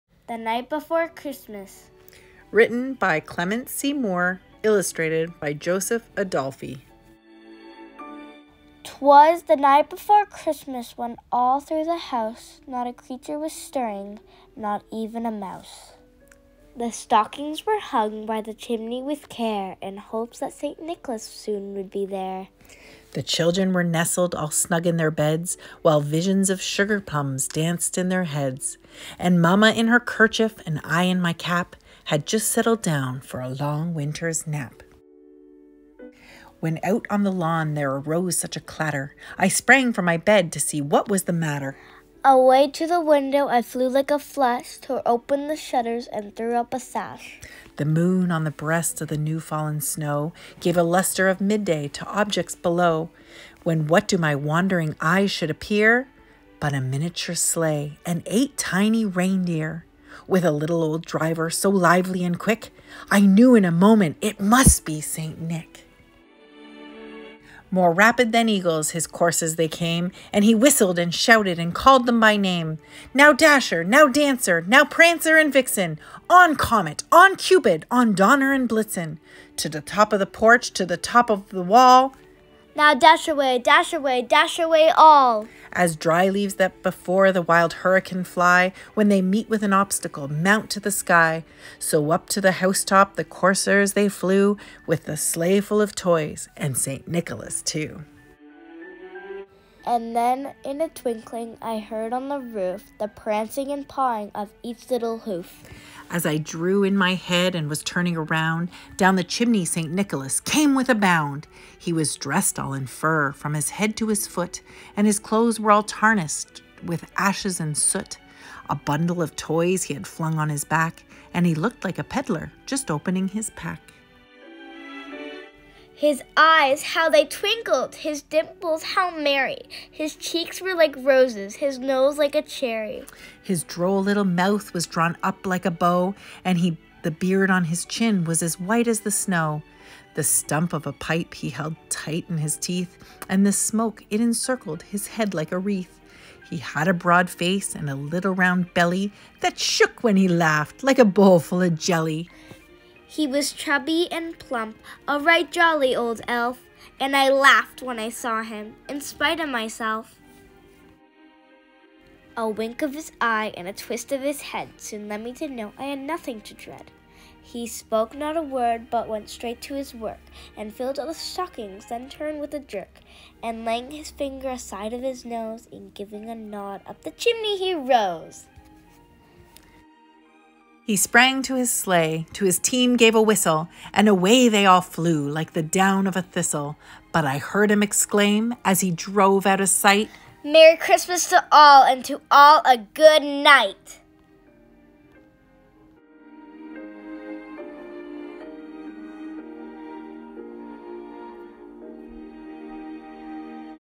Children's Storytime - The Night Before Christmas
St. Mark''s United Church
Children's Storytime - December 24, 2021
The Night Before Christmas
by Clement C. Moore
Illustrated by Joseph Adolphe